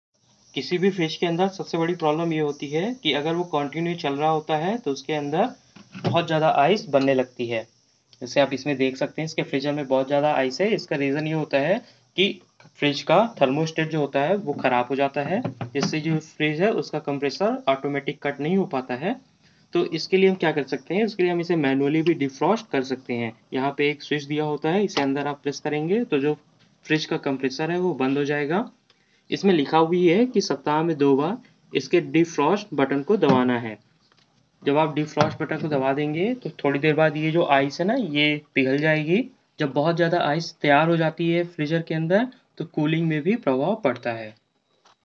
Fridge Freezer ice build up problem
Samsung fridge ke andar jyda ice banane lage to kaise usko defrost karte hai janiye is video me